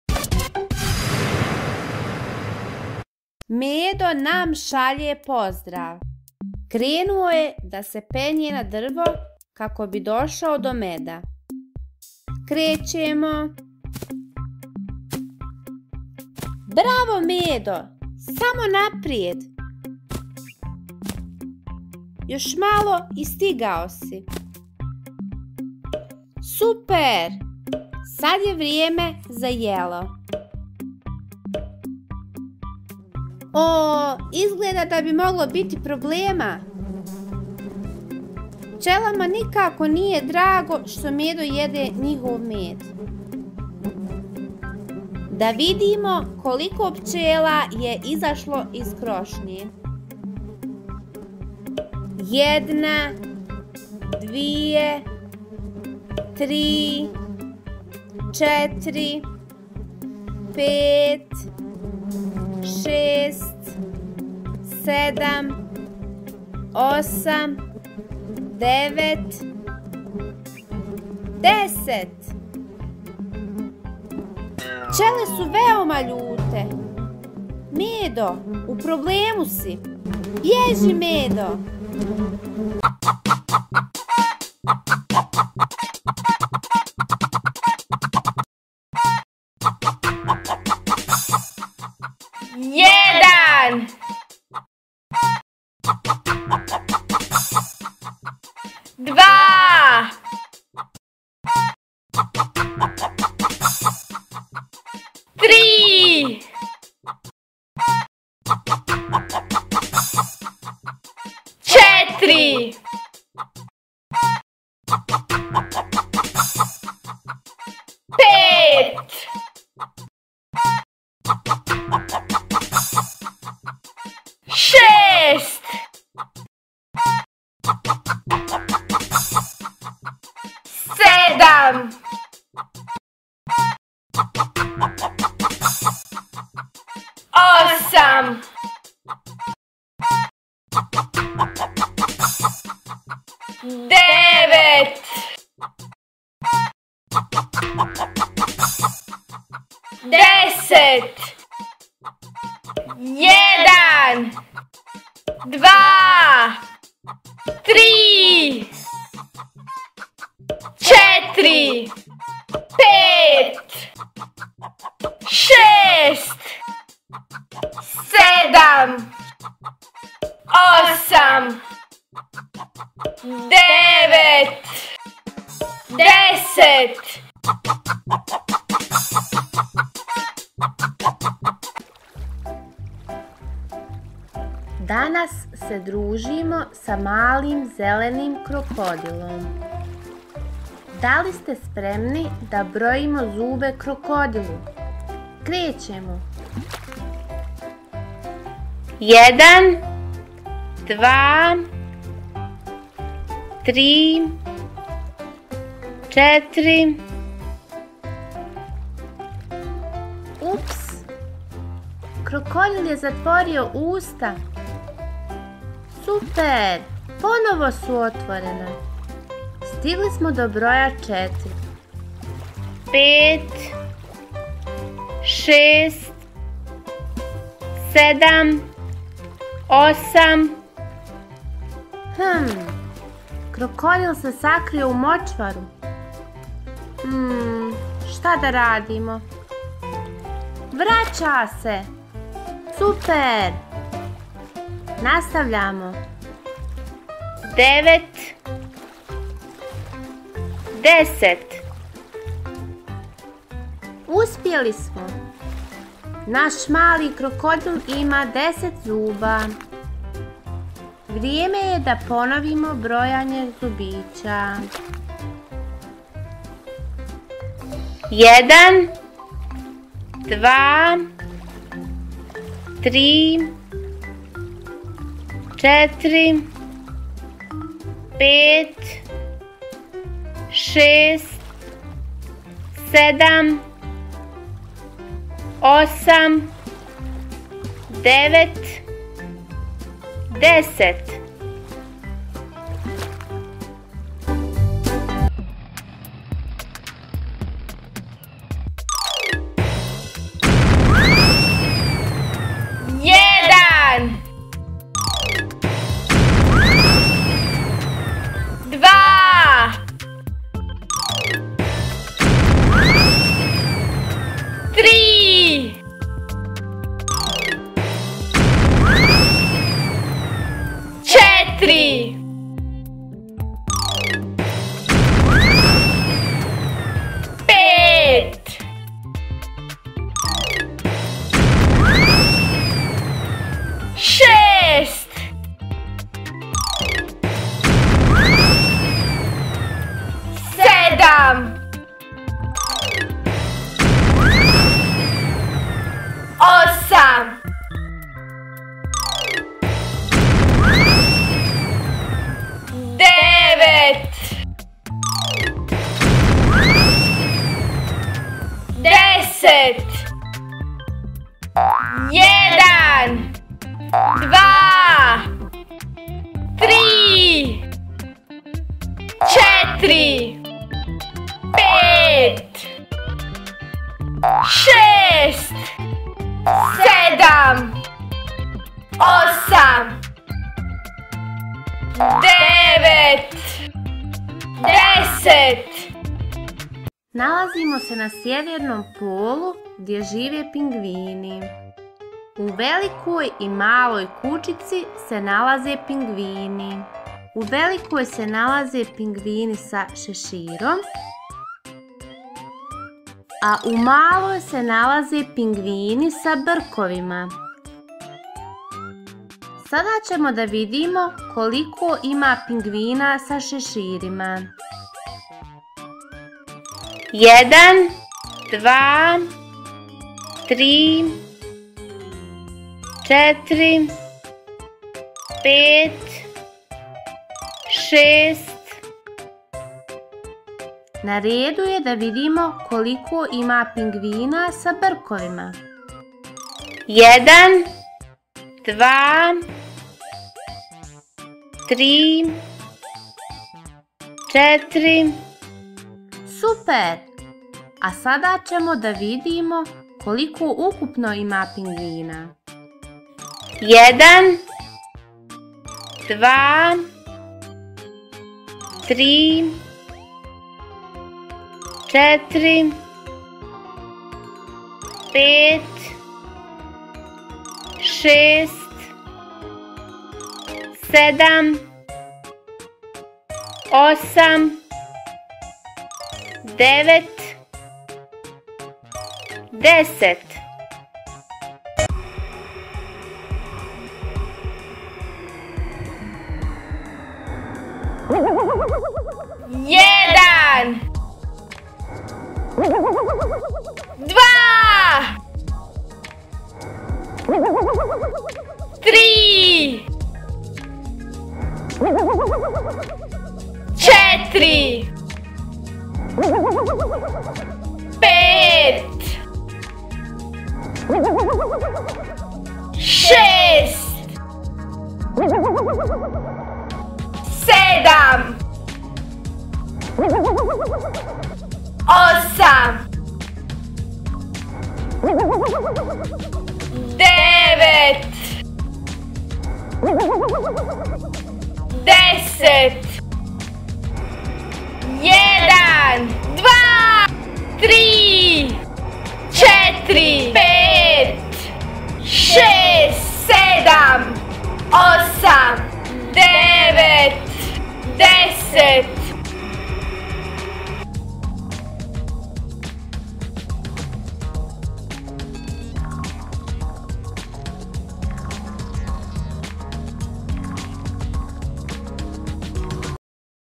UČIMO BROJEVE - na hrvatskom jeziku
Edukativni crtani za malu djecu. Učimo brojeve sa malim životinjama kao što su pilici, zeko, svemirci, medo, krokodil i pingvini. Crtani za djecu je namjenje govornom području za bosanski, srpski i hrvatski jezik.